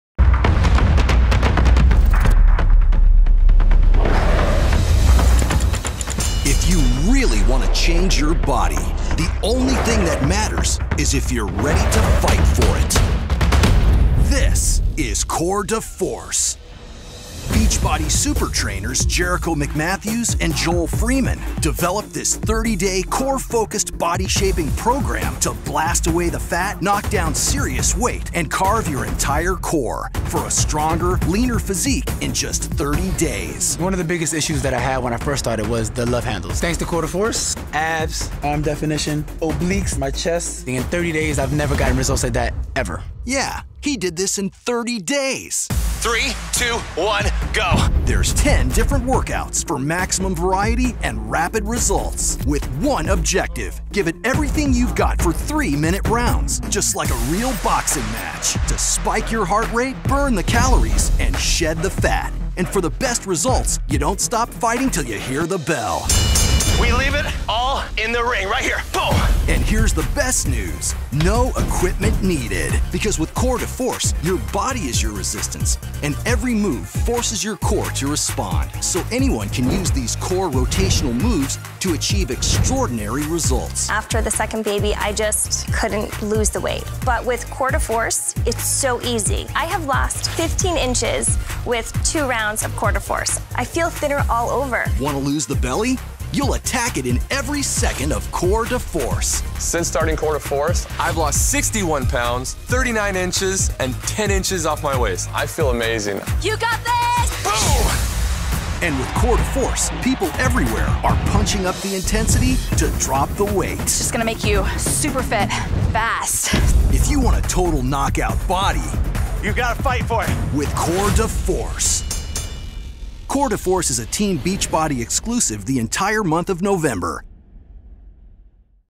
Core De Force - 30 Day Results Video
Beachbody's promotional video highlighting some of the test group participant results from this 30 day program!

Would you like to join us in our next challenge group? Fill out an application here !